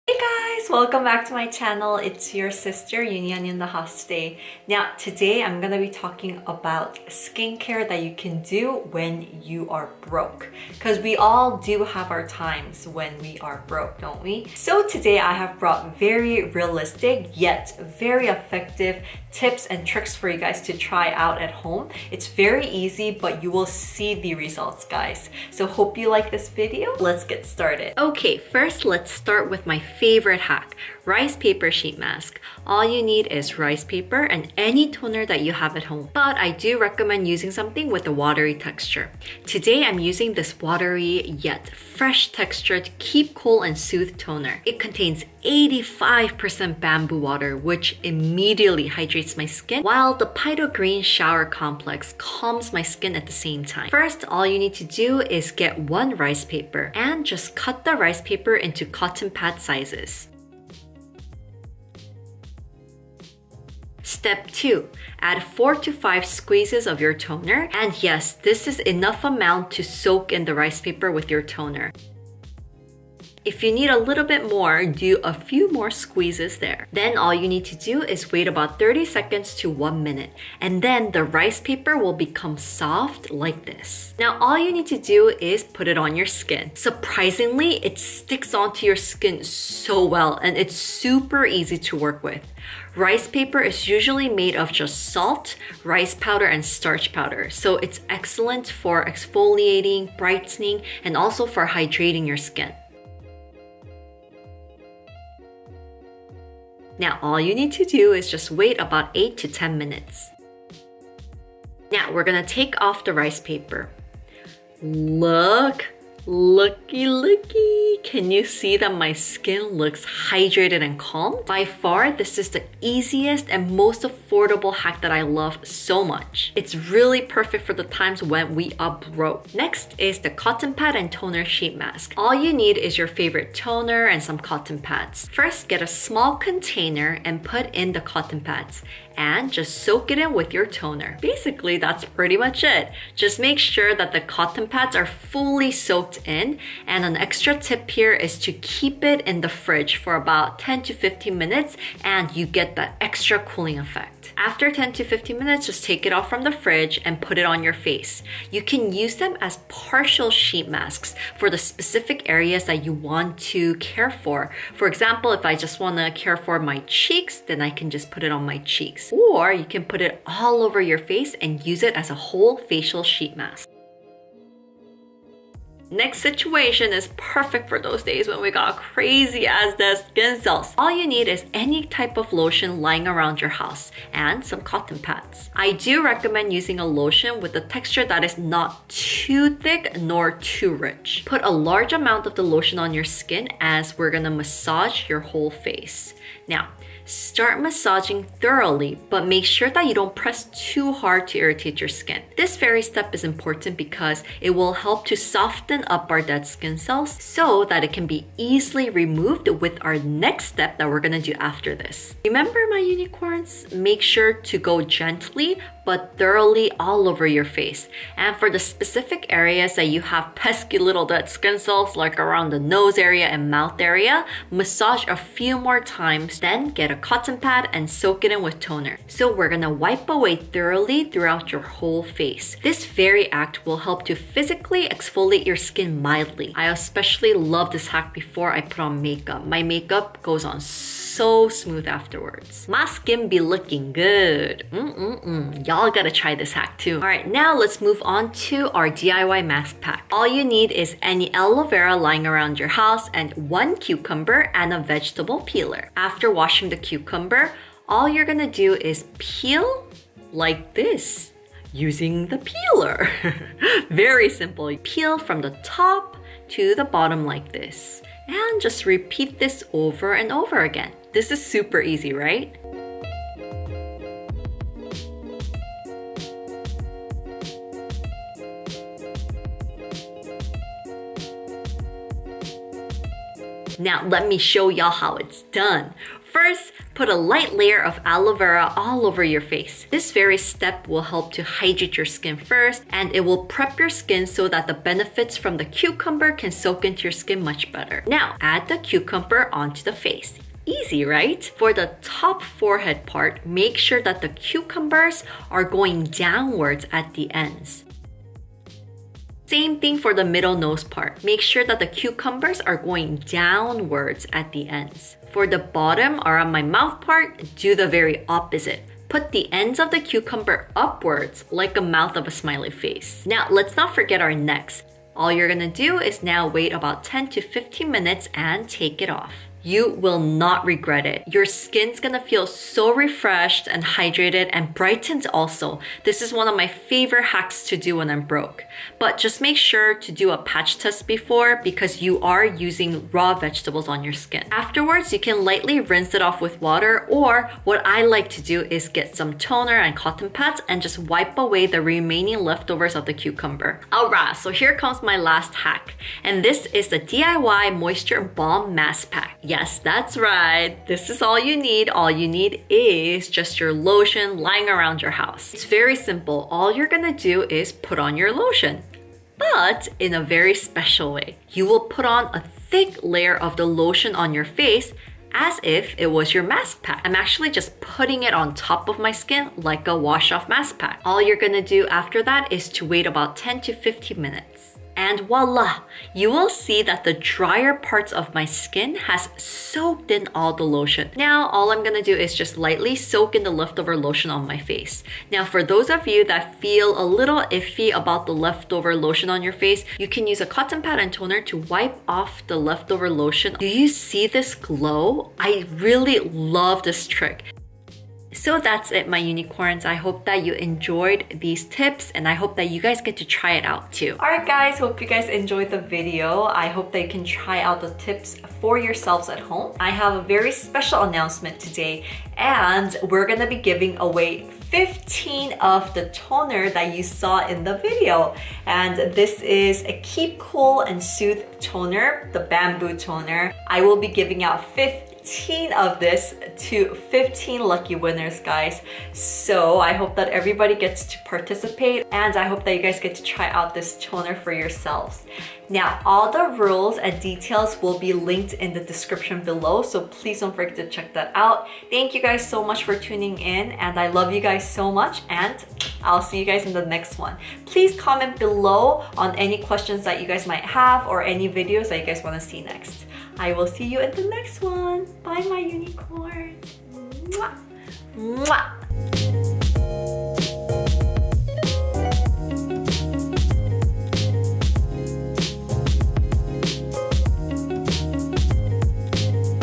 Tips 4 When We R Broke! Using products u have at home!
❐ FEATURED PRODUCTS IN VIDEO ❐

° KEEP COOL and Soothe Toner

° OXYGEN CEUTICALS Redness Soothing Aloe Vera Gel